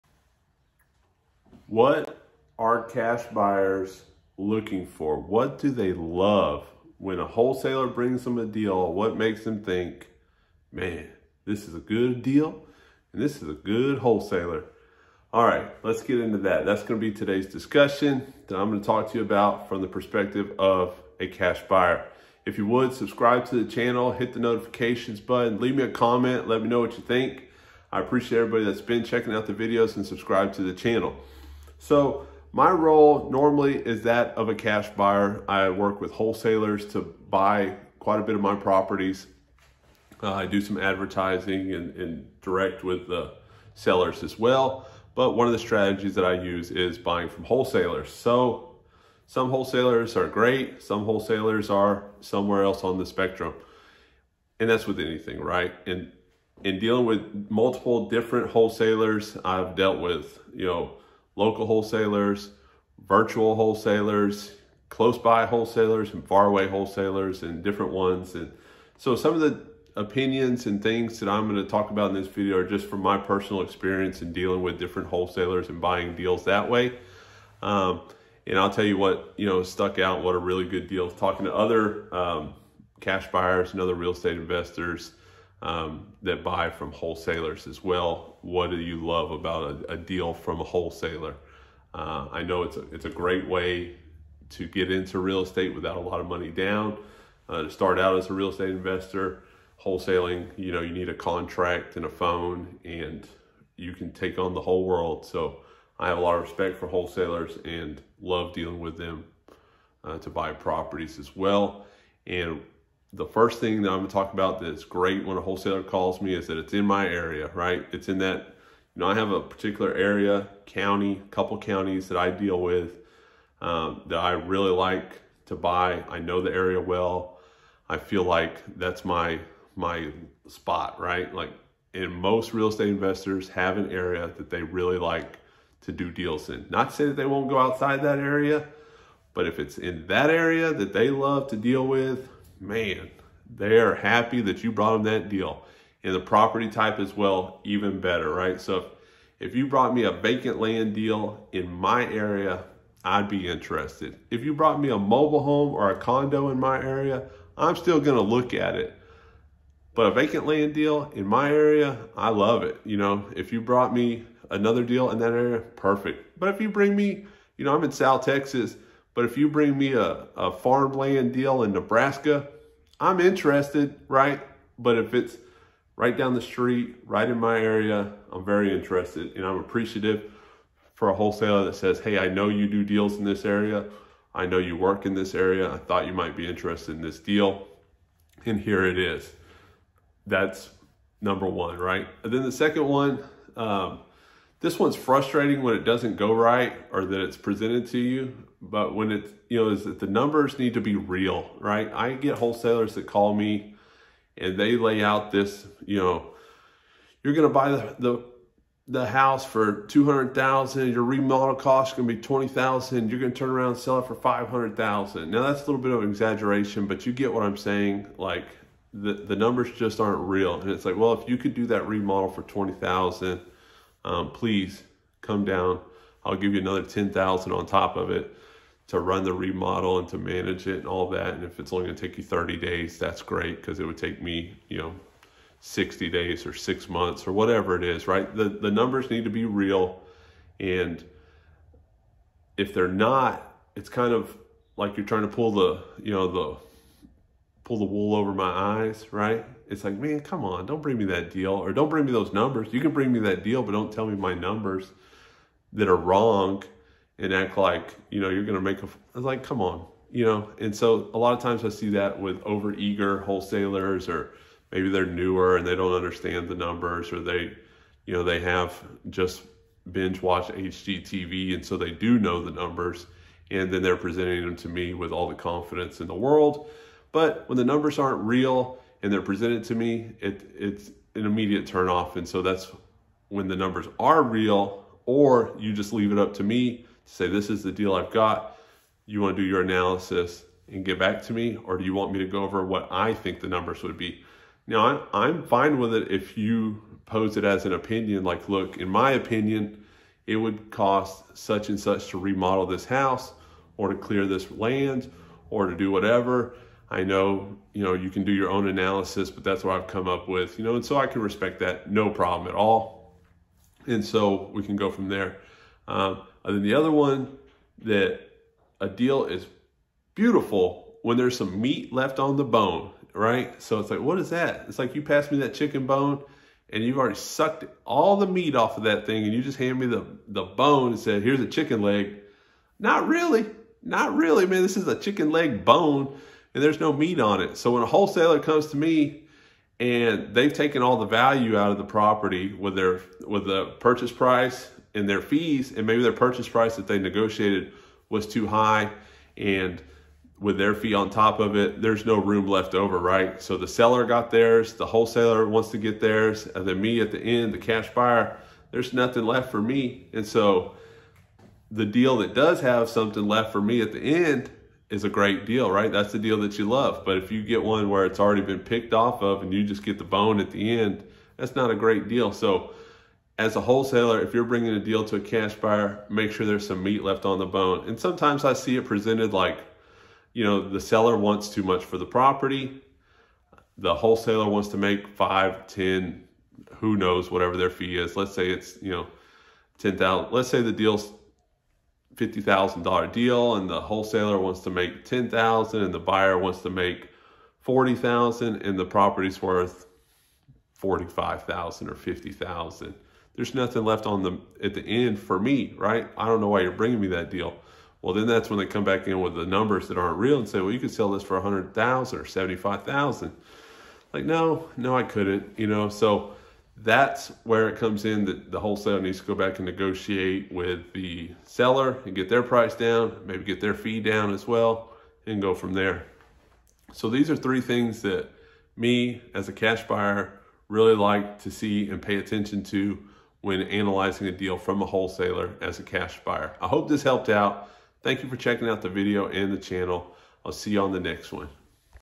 Three tips for dealing with Cash Buyers when wholesaling real estate
I want to share three tips that I think will be helpful for you in dealing with cash buyers if you're wholesaling real estate from the perspective of an active cash buyer.

thank you for checking out the video Three tips for dealing with Cash Buyers when wholesaling real estate